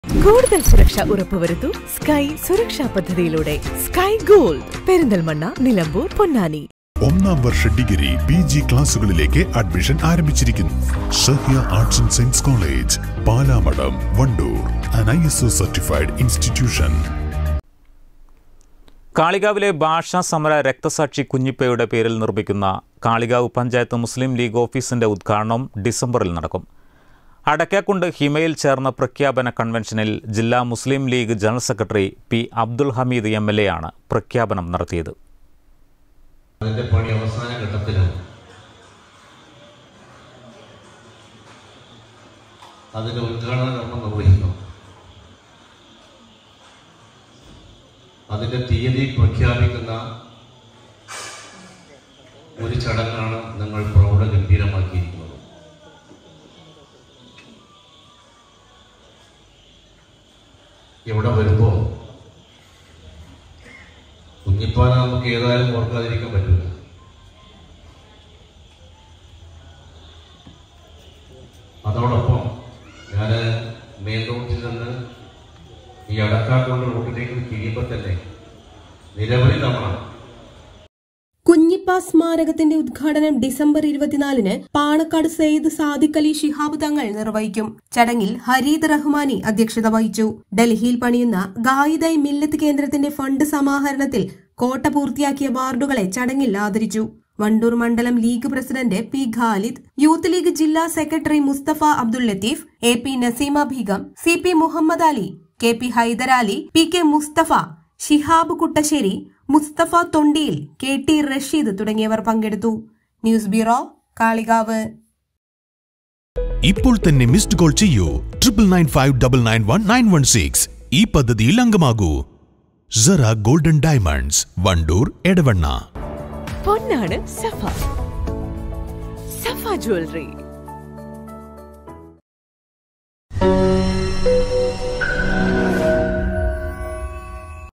കാളികാവ് പഞ്ചായത്ത് മുസ്ലിം ലീഗ് ഓഫീസിന്റെ ഉദ്ഘാടനം ഡിസംബറിൽ നടക്കും
പ്രഖ്യാപന കൺവൻഷനിൽ ജില്ലാ മുസ്ലിം ലീഗ് ജന: സെക്രട്ടറി പി. അബ്ദുൽ ഹമീദ്  MLA യാണ് പ്രഖ്യാപനം നടത്തിയത്.